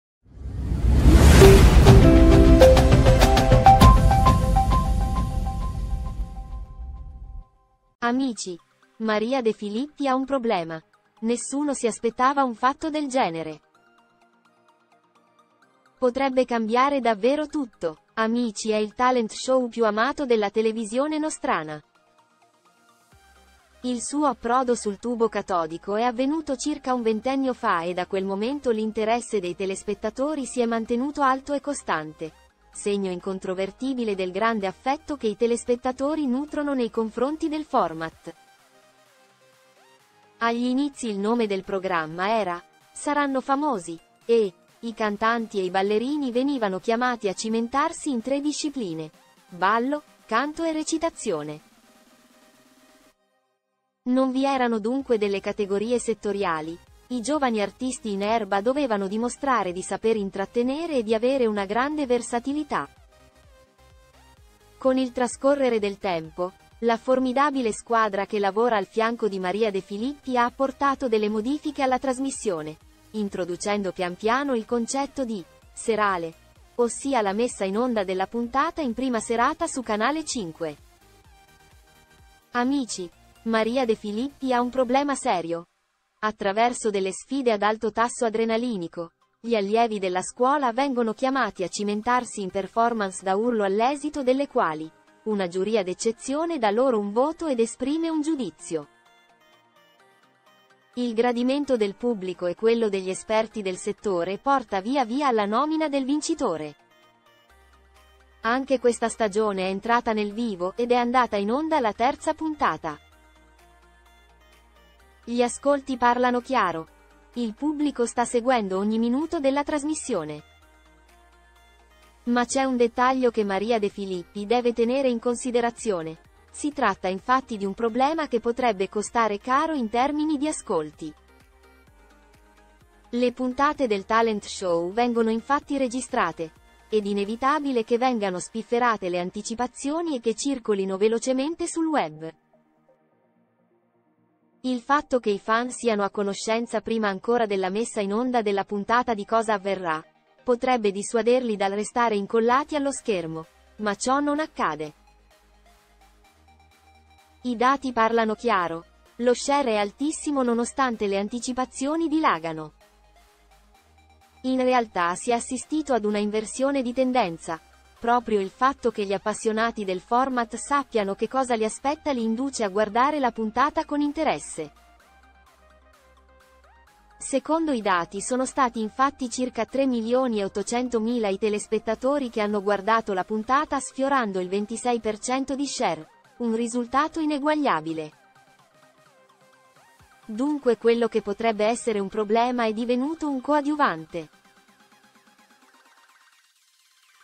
Maria De Filippi ha un problema serio con Amici nessuno poteva aspettarselo
Amici: Maria De Filippi ha un problema, nessuno si aspettava un fatto del genere. Potrebbe cambiare davvero tutto.

Amici è il talent show più amato della televisione nostrana. Il suo approdo sul tubo catodico è avvenuto circa un ventennio fa e da quel momento l’interesse dei telespettatori si è mantenuto alto e costante, segno incontrovertibile del grande affetto che i telespettatori nutrono nei confronti del format. Agli inizi il nome del programma era “Saranno famosi”e, i cantanti e i ballerini venivano chiamati a cimentarsi in tre discipline: ballo, canto e recitazione.